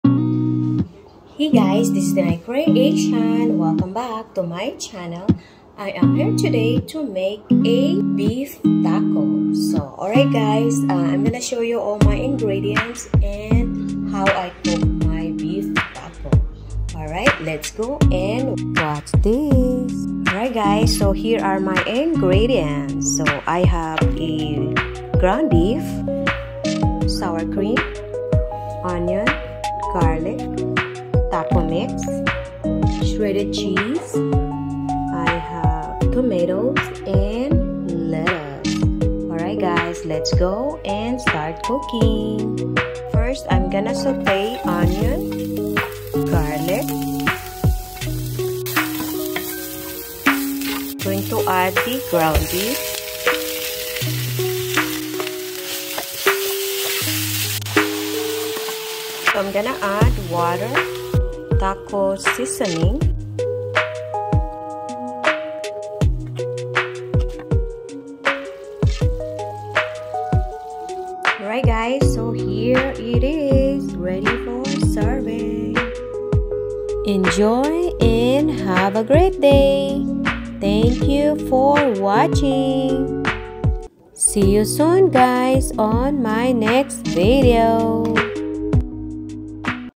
HOW I COOK MY BEEF TACO AND MAKE A BURRITO OR BEEF TACO SALAD
Easy home cooking at home with a delicious meal